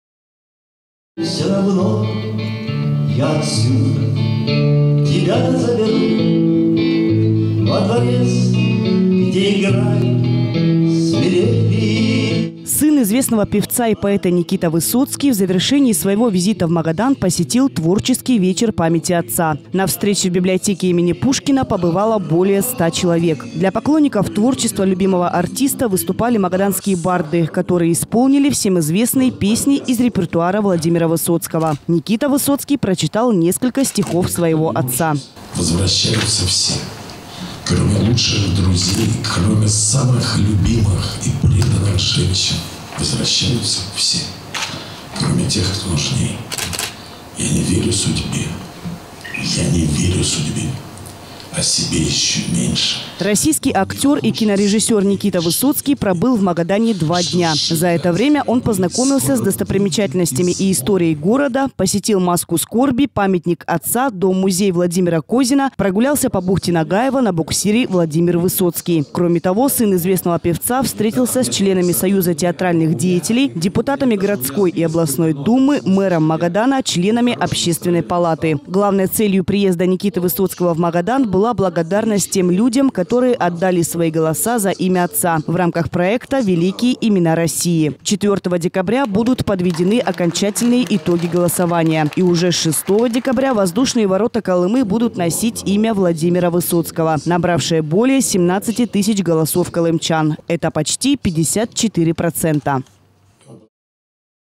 мтквидео Сын известного певца и поэта Никита Высоцкий посетил творческий вечер памяти отца
Сын известного певца и поэта Никита Высоцкий в завершение своего визита в Магадан, посетил творческий вечер памяти отца. На встрече, в библиотеке имени Пушкина побывало более 100 человек. Для поклонников творчества любимого артиста выступали магаданские барды, которые исполнили всем известные песни из репертуара Владимира Высоцкого. Никита Высоцкий прочитал несколько стихов своего отца